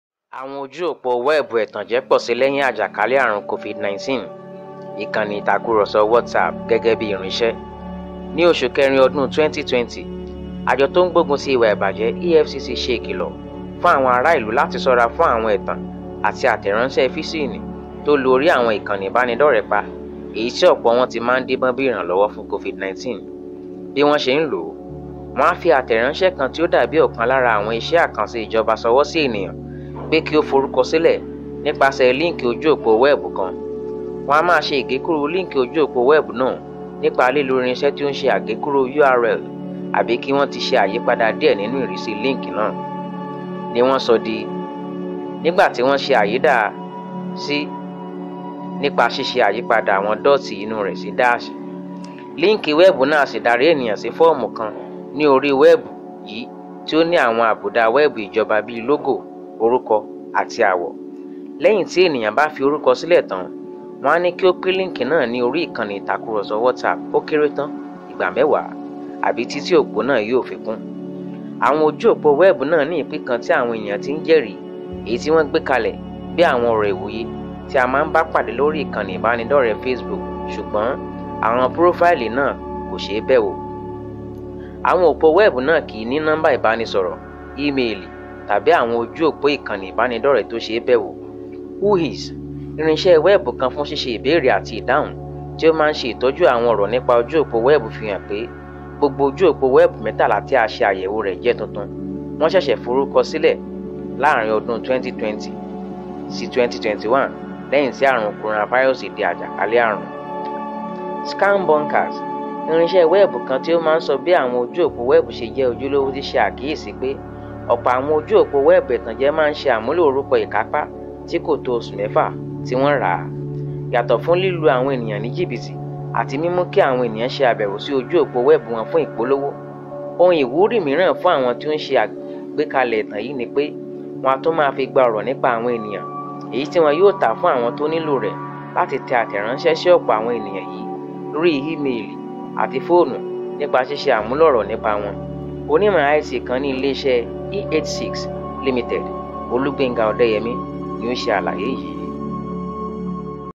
Àwọn ojú òpó wẹ́ẹ̀bù ẹ̀tànjẹ pọ̀ si lẹ́yìn àjàkálẹ̀ àrùn COVID-19: Ìkànnì Whatsapp gẹ́gẹ́ bí irinṣẹ́
YorubaFactCheck: Ní oṣù kẹrin ọdún 2020, àjọ tó ń gbógun ti ìwà ìbàjẹ́, EFCC ṣe ìkìlọ̀ fún àwọn ará ìlú láti sọ́ra fún àwọn ẹ̀tàn àti àtẹ̀ráńṣẹ́ físíìnì tó lu orí àwọn ìkànnì ìbánidọ́rẹ̀ẹ́ pa, èyí tí ọ̀pọ̀ wọn ti ń díbọ́n bí ìrànlọ́wọ́ fún COVID-19.

Bí wọ́n ṣe ń lò ó: Wọn á fi àtẹ̀ráńṣẹ́ kan tí ó dà bí ọkàn lára àwọn iṣẹ́ àkànṣe ìjọba sọwọ́ sí ènìyàn pé kí ó forúkọsílẹ̀ nípasẹ̀ líǹkì ojú òpó wẹ́ẹ̀bù kan.

Fun alaaye, s'abewo si: FactCheckHub.com

Atúmọ̀-èdè: Simeon Onaolapo
Òǹkọ̀wé: Bamas VicToria